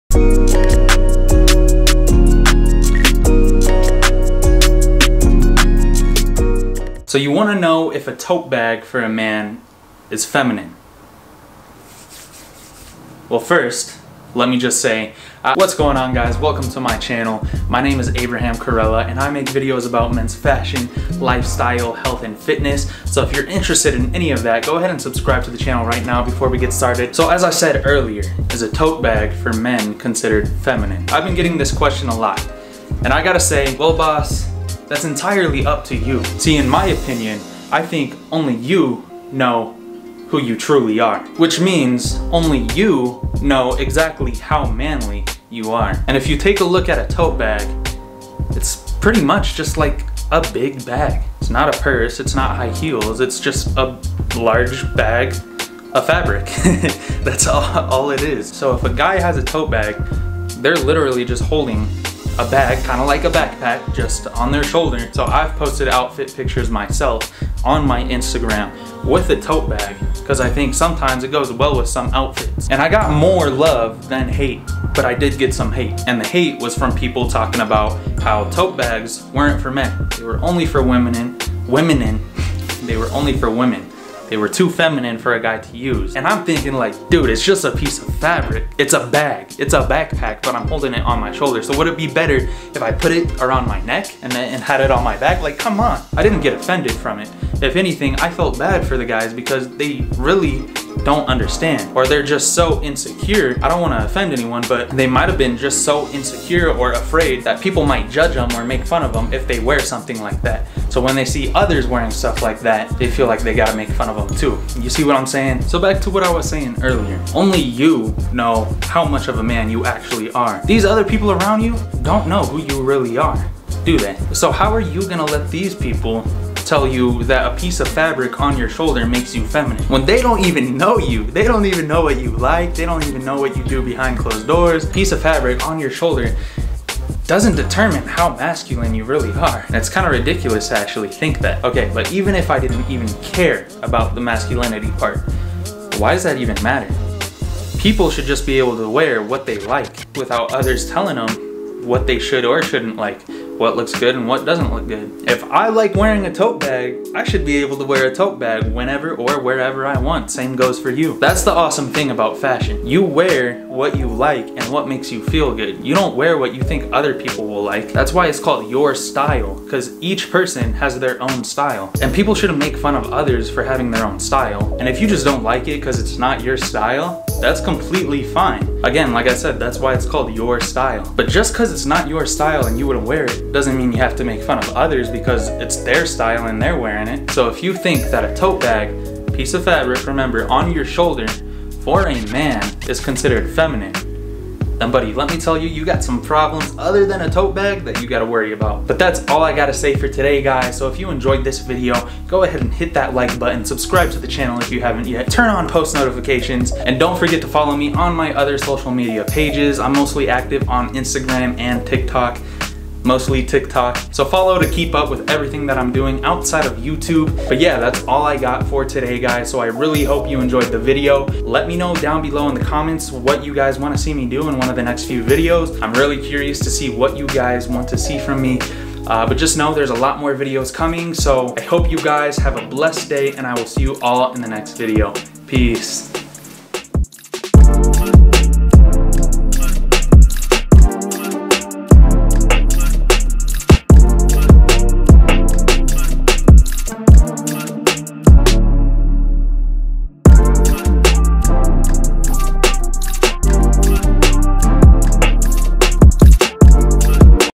Are Tote Bags For Men Feminine?
I've been getting this same question from alot of people lately. Everyone keeps asking me, "are tote bags for men considered feminine?" I talk about my views on the topic and explain why I think it's ridiculous to think such a thing. If you don't like it then you don't have to wear it. Wear what you like but don't make fun of others for what they like.

I make videos about men's fashion, fitness, health and lifestyle.